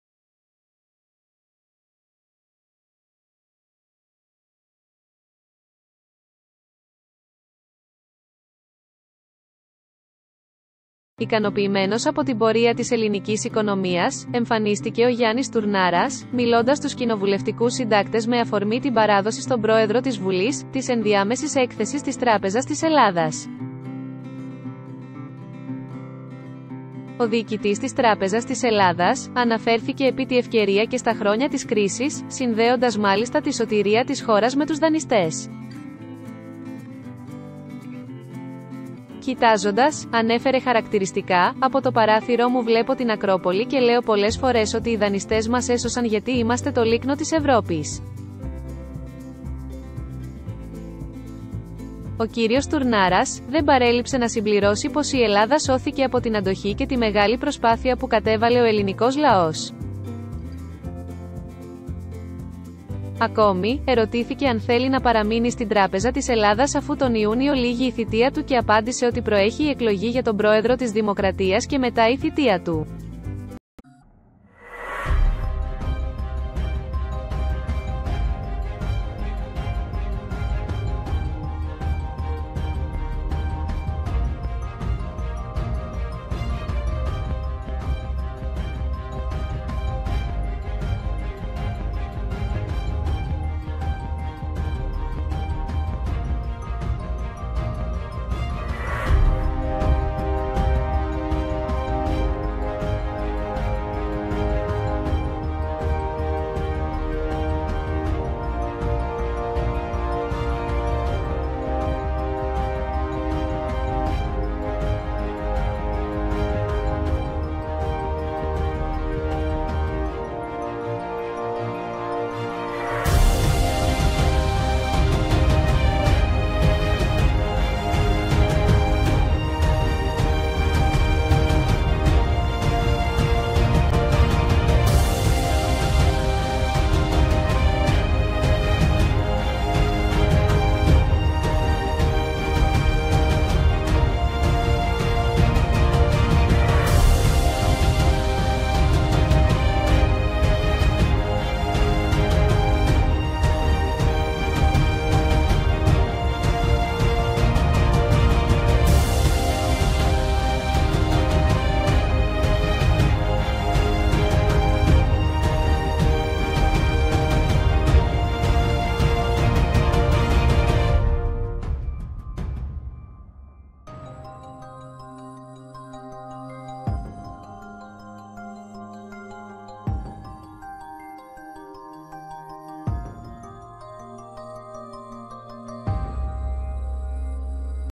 ✅ Στουρνάρας: «Βλέπω την Ακρόπολη από το παράθυρό μου και λέω: Οι δανειστές μας έσωσαν»
✍ Ικανοποιημένος από την πορεία της ελληνικής οικονομίας, εμφανίστηκε ο Γιάννης Στουρνάρας, μιλώντας στους κοινοβουλευτικούς συντάκτες με αφορμή την παράδοση στον πρόεδρο της Βουλής, της ενδιάμεσης έκθεσης της Τράπεζας της Ελλάδας. .Ο διοικητής της Τράπεζας της Ελλάδας, αναφέρθηκε επί τη ευκαιρία και στα χρόνια της κρίσης, συνδέοντας μάλιστα την σωτηρία της χώρας με τους δανειστές. «Κοιτάζοντας», ανέφερε χαρακτηριστικά, «από το παράθυρό μου βλέπω την Ακρόπολη και λέω πολλές φορές ότι οι δανειστές μας έσωσαν γιατί είμαστε το λίκνο της Ευρώπης». Ο κ. Στουρνάρας, δεν παρέλειψε να συμπληρώσει πως η Ελλάδα σώθηκε από την αντοχή και τη μεγάλη προσπάθεια που κατέβαλε ο ελληνικός λαός. Ακόμη, ερωτήθηκε αν θέλει να παραμείνει στην Τράπεζα της Ελλάδας -αφού τον Ιούνιο λήγει η θητεία του- και απάντησε ότι προέχει η εκλογή για τον Πρόεδρο της Δημοκρατίας και μετά η θητεία του.  ...